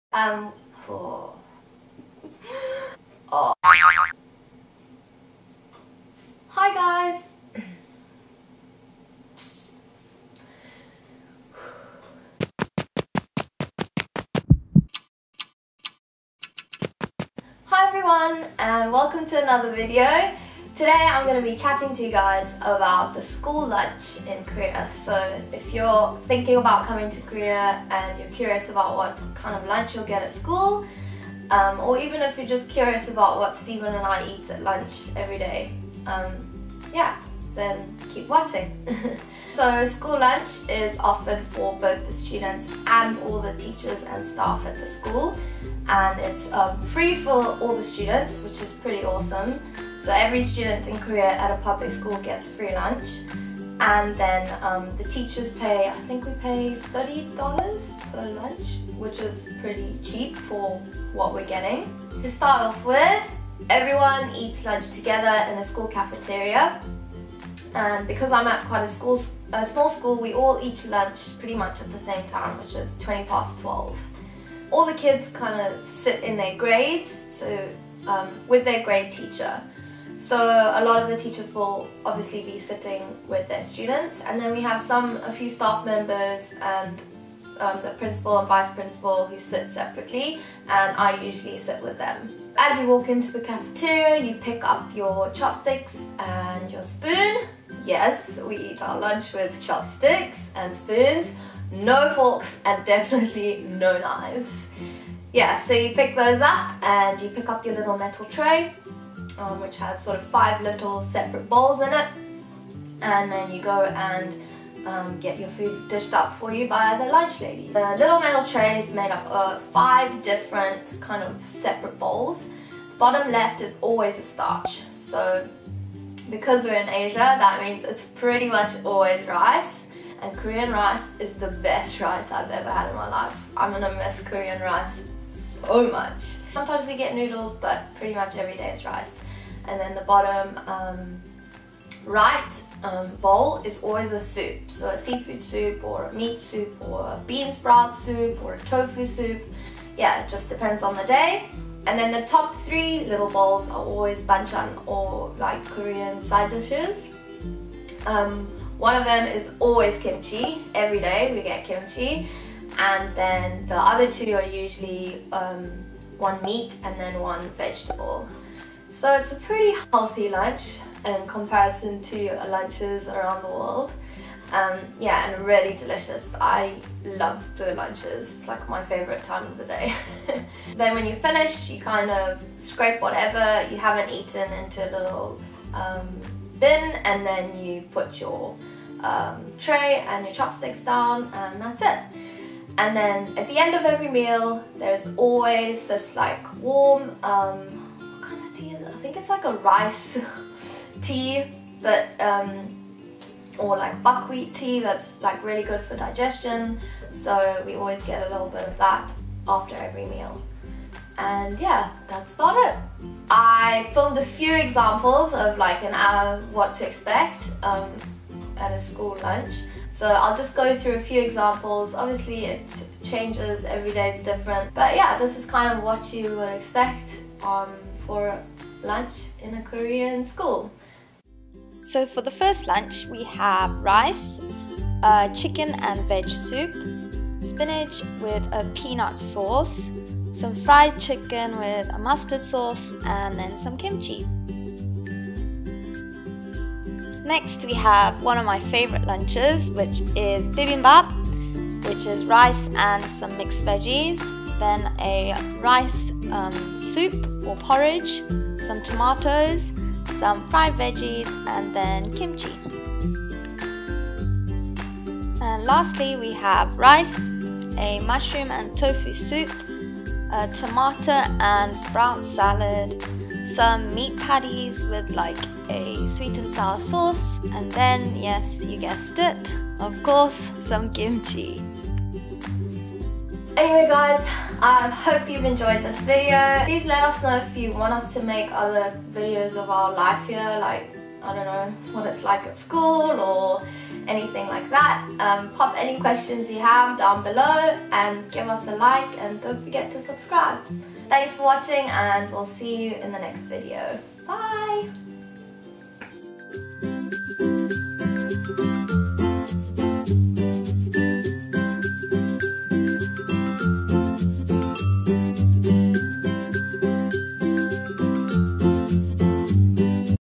School Lunch in South Korea
A little look into what we eat for lunch in a Korean Elementary School. One of our favourite things about Korea is the food, and the school food is no exception. It's cheap, delicious and relatively healthy. So, its no wonder that lunchtime is one of the best parts of our school day :)

OUR GEAR :

👣Big Camera
👣GoPro (The BEST)
👣Small Camera (Great for vlogging)
👣“Action” Camera
👣Bag (Best quality. Love it!)